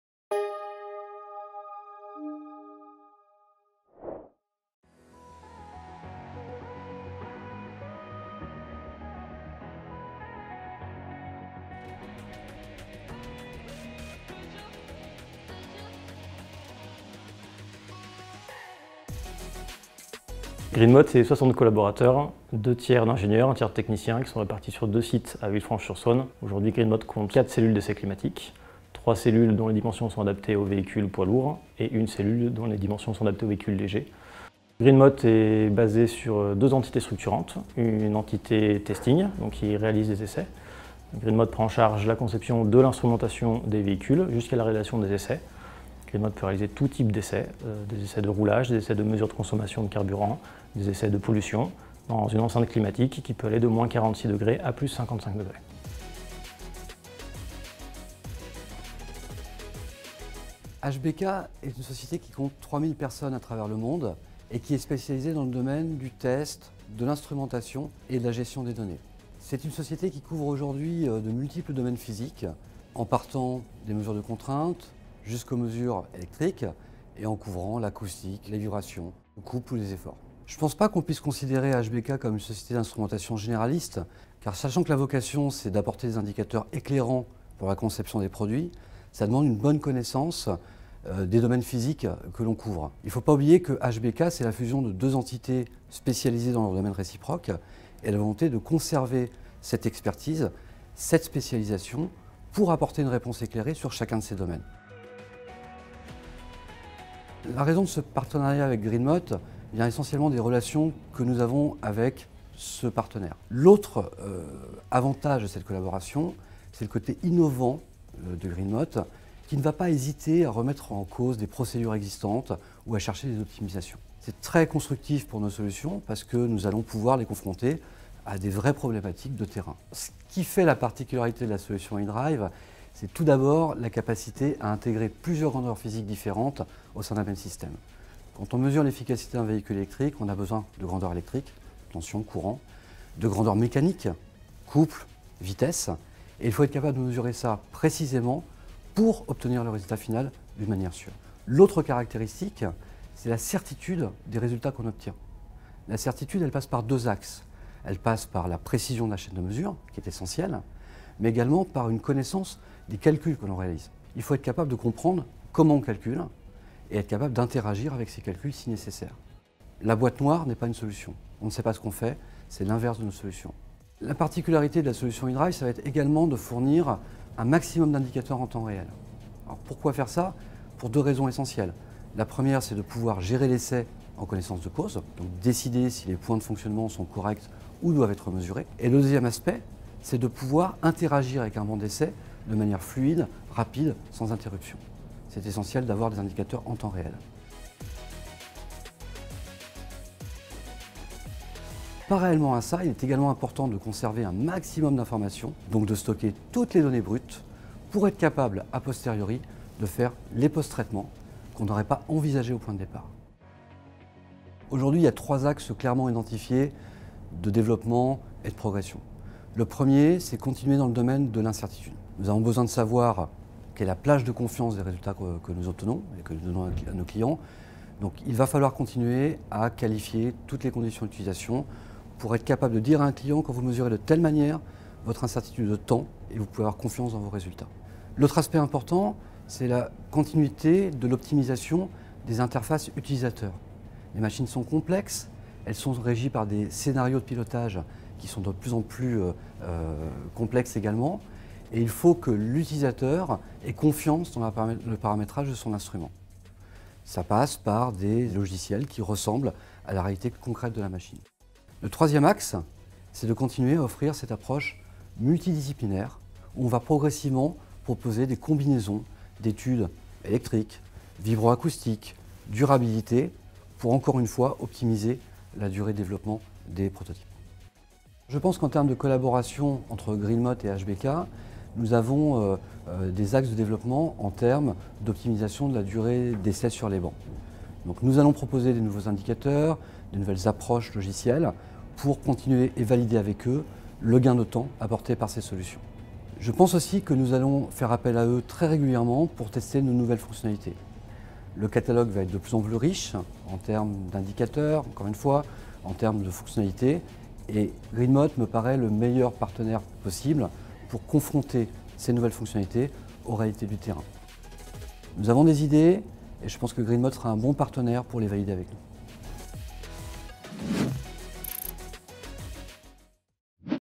Electric power management - A successful partnership with GreenMot for electric vehicle development
We have ideas and I think Greenmot will be the right partner to validate them with us.”
HBK - Hottinger Brüel & Kjær and Greenmot -  partners for more than 10 years

In this interview, Jonathan MASY-ROUGIER, Head of Operations, GreenMot - Thomas LEDAN, Metrology & Maintenance Depart. Manager, GreenMot and Philippe Potereau, Business Developer Manager eDrive and Power Analysis, HBK address a major subject for electric vehicle testing : the in-situ instrumentation and solutions which makes possible the collection of precise, reliable and repeatable data.

HBK's measurement solutions allow Greenmot to face a main issue: to get synchronized data and data that are in the same measurement environment.
Moreover, the partnership with the vehicle testing center gives to HBK the opportunity to compare these new features to the on-the-ground realities.